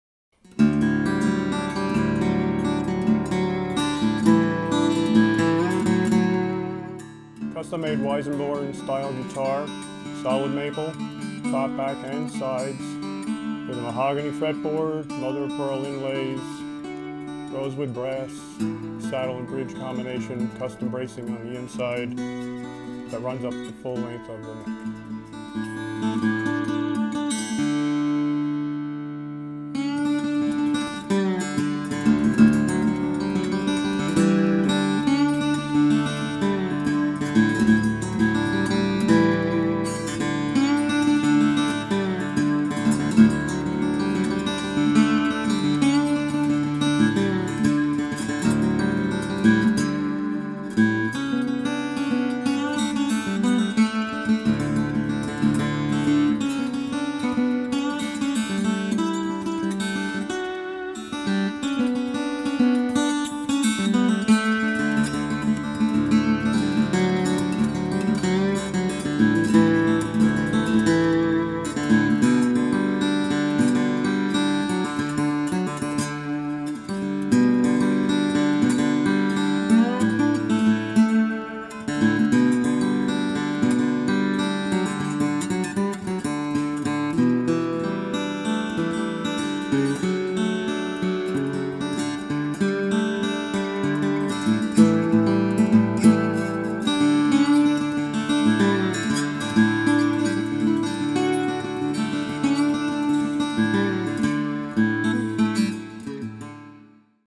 Custom Weissenborn Solid Maple Acoustic Steel Guitar
Solid maple top, back, and sides. Mahogany headplate and fretboard. MOP inlays. Cream bindings.Rosewood bridge, solid brass saddle. Custom maple bracing. Jack Stepick, Luthier